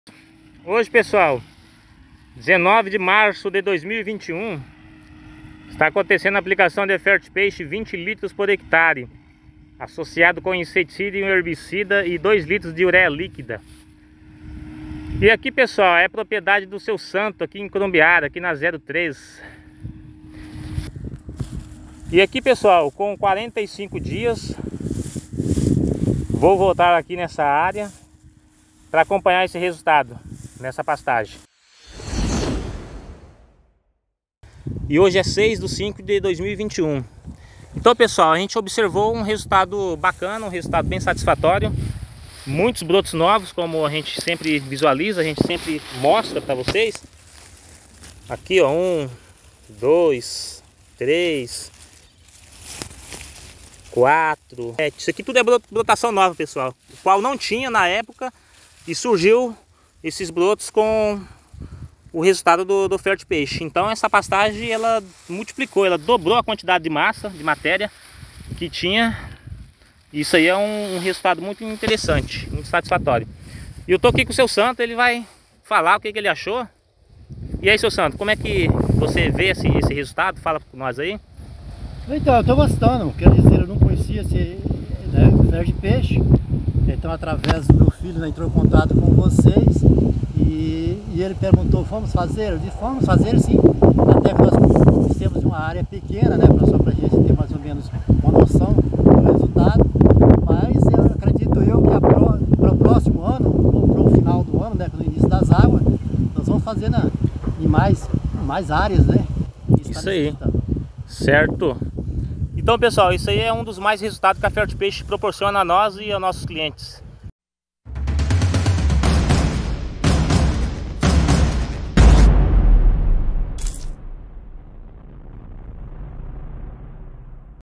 Adubação no Capim Brachiaria - Resultado - Adubo Orgânico - Herbicida - Uréia - Ferti-Peixe
Representante: David Arão
Pecuarista: Santo

Resultado no Capim Brachiaria com aumento da quantidade de massa,  grande brotação e perfilhamento.